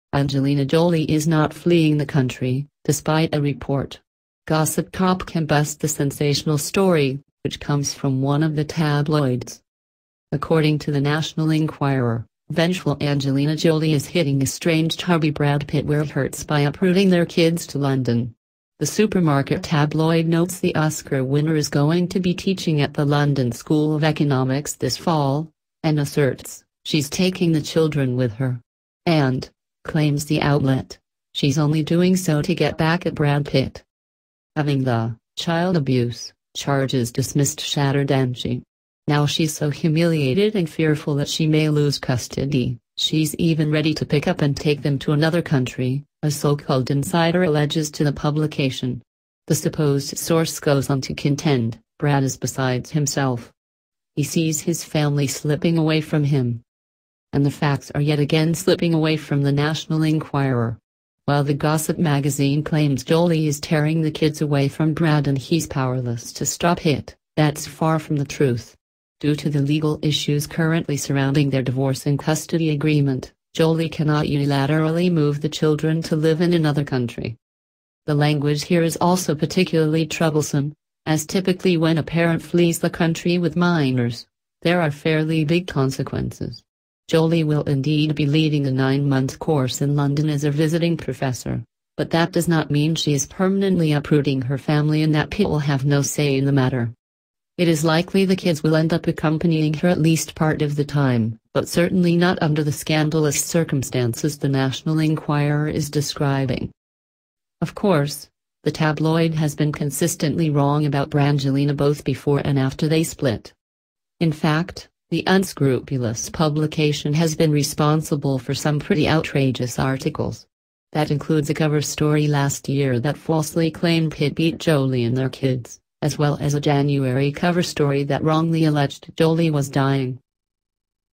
Angelina Jolie NOT Fleeing Country, Despite Report
Angelina Jolie is NOT “fleeing the country,” despite a report. Gossip Cop can bust the sensational story, which comes from one of the tabloids.

According to the National Enquirer, “Vengeful Angelina Jolie is hitting estranged hubby Brad Pitt where it hurts by uprooting their kids to London!” The supermarket tabloid notes the Oscar winner is going to be teaching at the London School of Economics this fall, and asserts, “she’s taking the children with her.” And, claims the outlet, she’s only doing so “to get back” at Brad Pitt.

“Having the [child abuse] charges dismissed shattered Angie. Now she’s so humiliated and fearful that she may Alose custody, she’s even ready to pick up and take them to another country,” a so-called “insider” alleges to the publication. The supposed source goes on to contend, “Brad is besides himself. He sees his family slipping away from him!”

And the facts are yet again slipping away from the National Enquirer. While the gossip magazine claims Jolie is “tearing” the kids “away from Brad” and he’s “powerless to stop it,” that’s far from the truth. Due to the legal issues currently surrounding their divorce and custody agreement, Jolie cannot unilaterally move the children to live in another country.

The language here is also particularly troublesome, as typically when a parent “flees the country” with minors, there are fairly big consequences. Jolie will indeed be leading a nine-month course in London as a visiting professor, but that does not mean she is permanently “uprooting” her family and that Pitt will have no say in the matter. It is likely the kids will end up accompanying her at least part of the time, but certainly not under the scandalous circumstances the National Enquirer is describing.

Of course, the tabloid has been consistently wrong about Brangelina both before and after they split. In fact, the unscrupulous publication has been responsible for some pretty outrageous articles. That includes a cover story last year that falsely claimed Pitt beat Jolie and their kids, as well as a January cover story that wrongly alleged Jolie was dying.